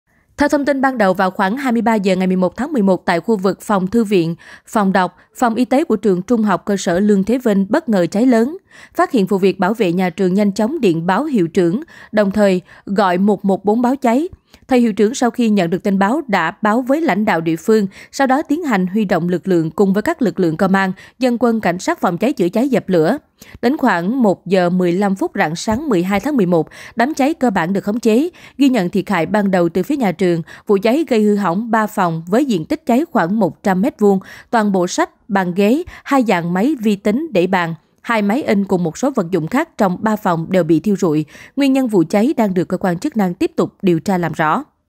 Trường Học Ở Quảng Nam Bốc Cháy Ngùn Ngụt Trong Đêm, Sách Vở Và Nhiều Tài Sản Bị Thiêu Rụi
SKĐS | Tối muộn ngày 11/11, một ngọn lửa đã bất ngờ bùng phát, nuốt chửng 3 căn phòng của một trường THCS tại Quảng Nam khiến nhiều tài sản bị thiêu rụi. Hiện cơ quan chức năng đang tiếp tục điều tra, làm rõ vụ việc.

Mời quý vị và các bạn xem thêm: Người Con Trai Duy Nhất Tiết Lộ Mảnh Đất Tranh Chấp Chưa Chia Cho Ai